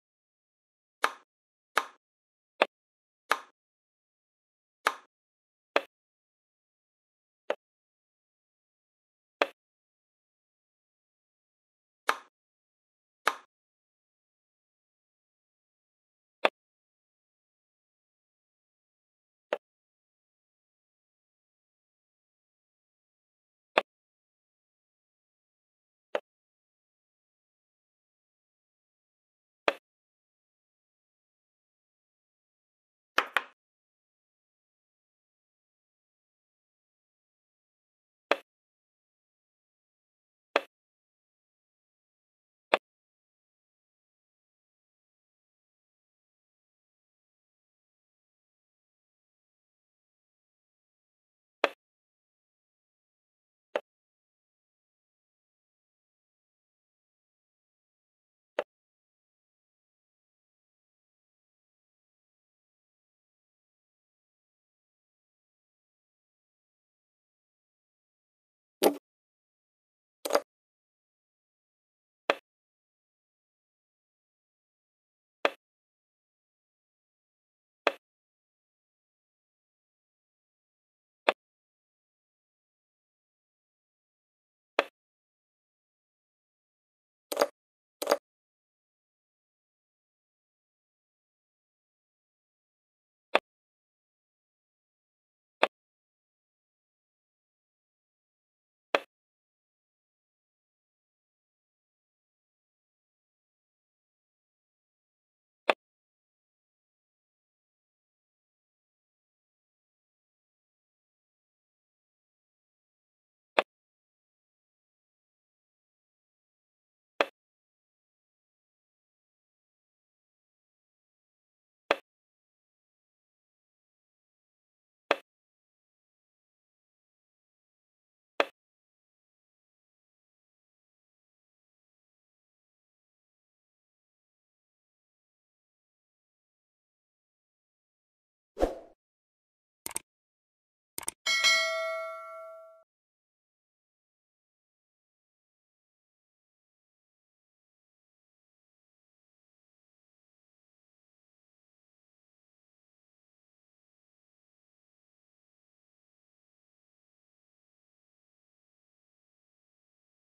Series 34 - Slav Defense || 6.Bd3
The Slav Defense is a chess opening that begins with the moves:
1. d4 d5
2. c4 c6
The Slav is one of the primary defenses to the Queen's Gambit. '